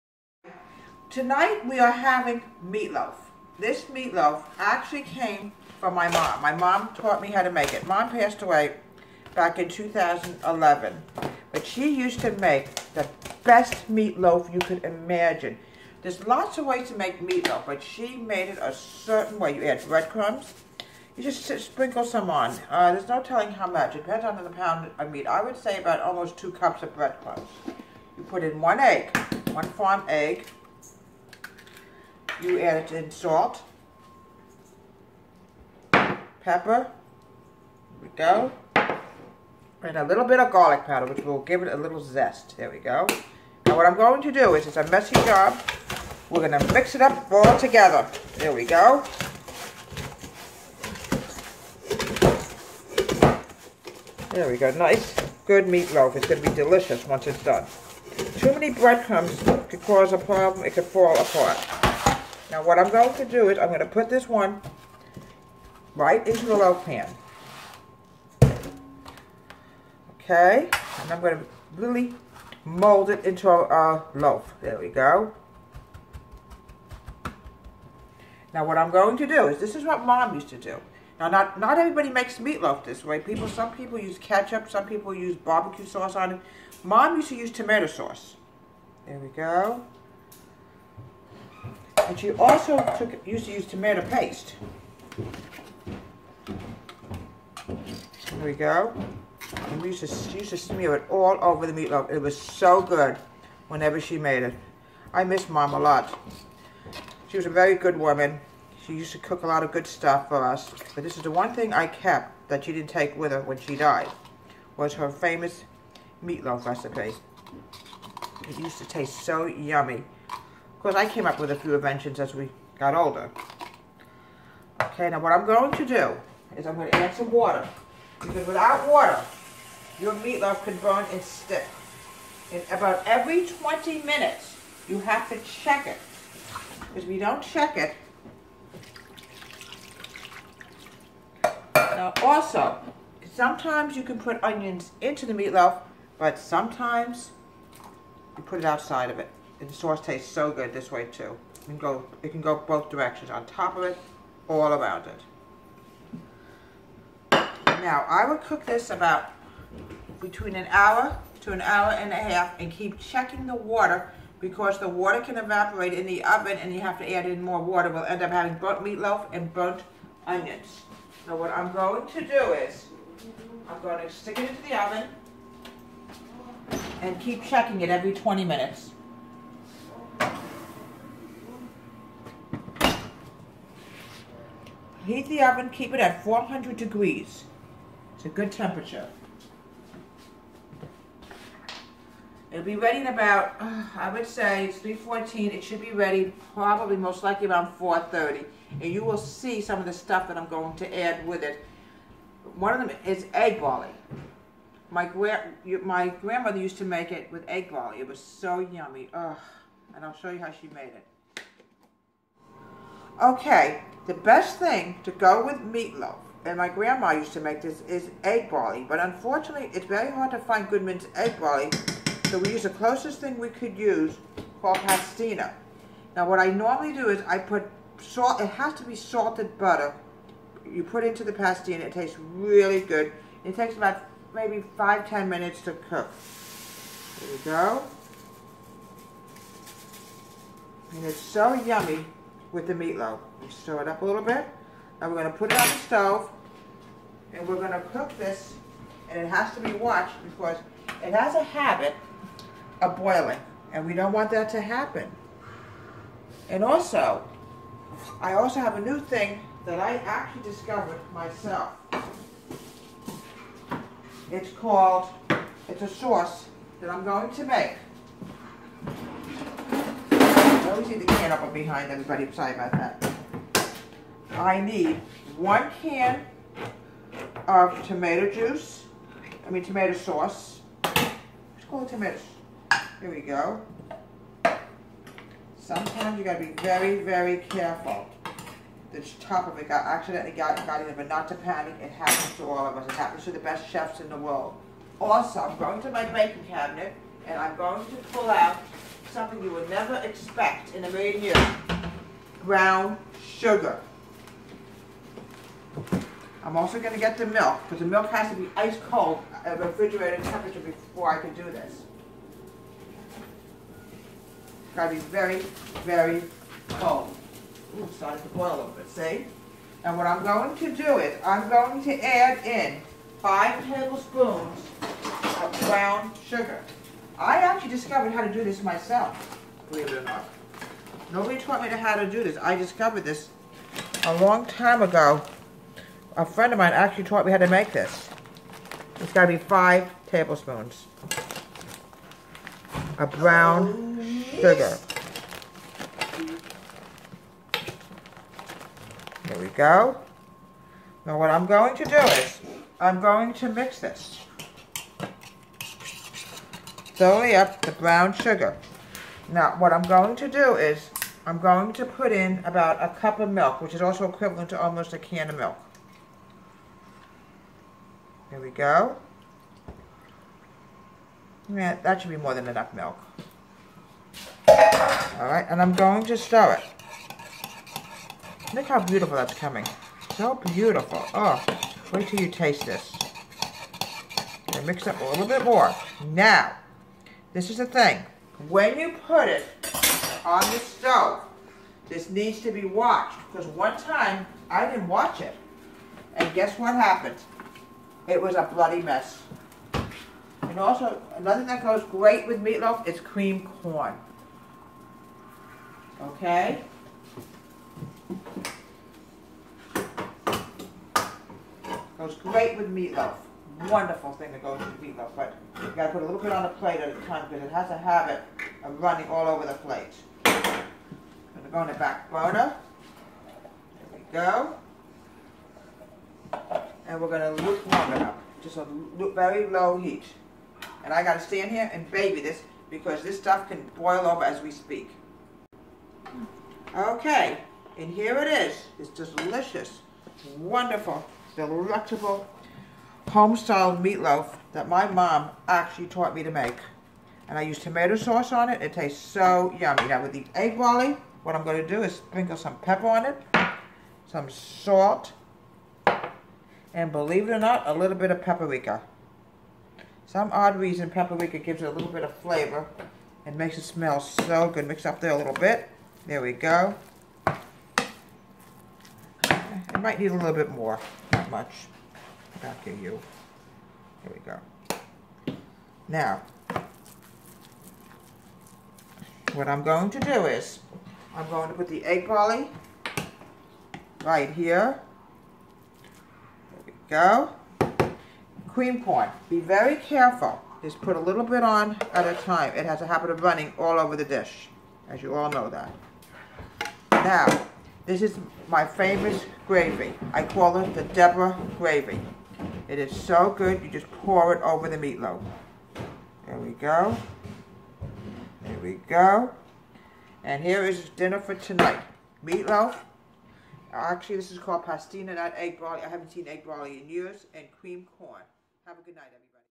Classic Meatloaf!
Today we make an old favorite. MEATLOAF!